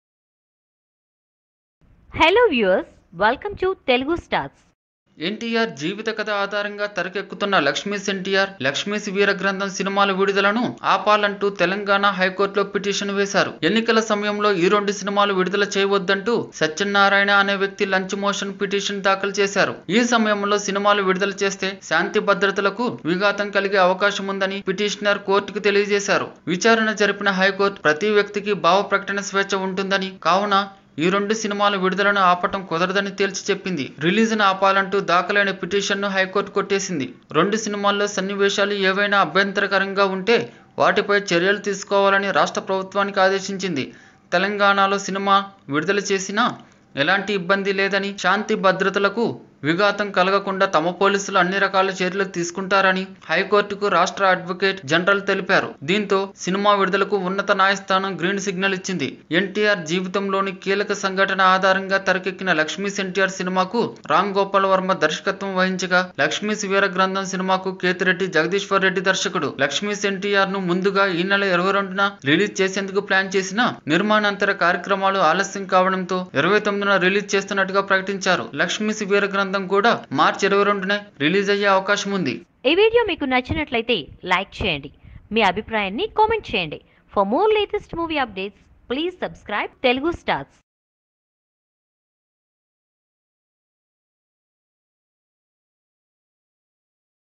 High Court Gives Green Signal To Lakshmi'S NTR Movie Release | RGV | NTRtrueSTORY | Yagna Shetty
High Court Gives Green Signal To Lakshmi'S NTR Movie Release . RGV . NTRtrueSTORY . Telugu Stars .Telangana High Court has given Green Signal for the release of Ram Gopal Varma’s controversial film Lakshmi’s NTR. A person filed a lunch motion petition in Telangana High Court asking to stop the release of the film until the elections since it can pose law and order issues in Andhra Pradesh.High Court struck down the petition much to the relief of the makers. The movie will be placed for censor tomorrow and if it is manages to get the Censor Certification, it will see the light. The movie was earlier planned for March 22nd release but now is pushed to March 29th.

లక్ష్మీస్‌ ఎన్టీఆర్‌ రిలీజ్ ఆపటం కుదరదని హైకోర్టు తేల్చి చెప్పింది